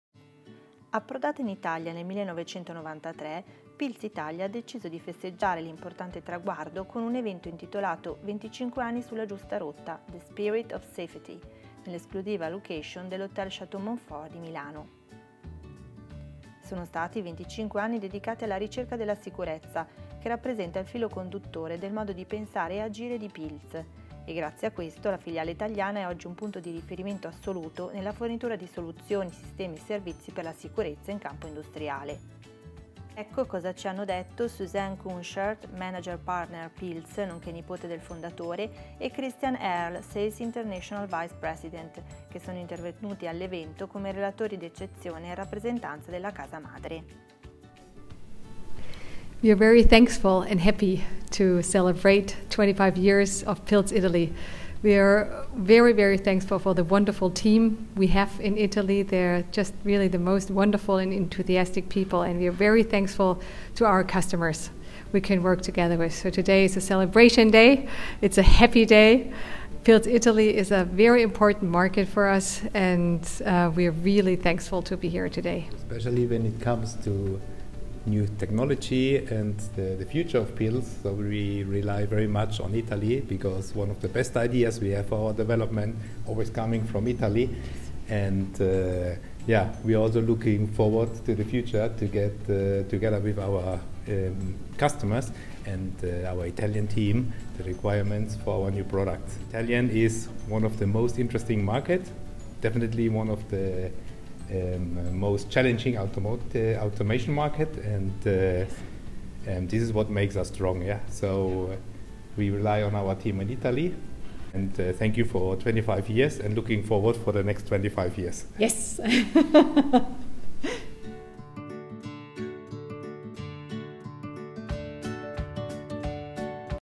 Pilz 25 anni
Video intervista di Automazione Oggi a Susanne Kunschert, Manager Partner Pilz, e Christian Erles, Sales International Vice President, intervenuti come relatori d’eccezione in occasione dei festeggiamenti per i 25 anni di Pilz Italia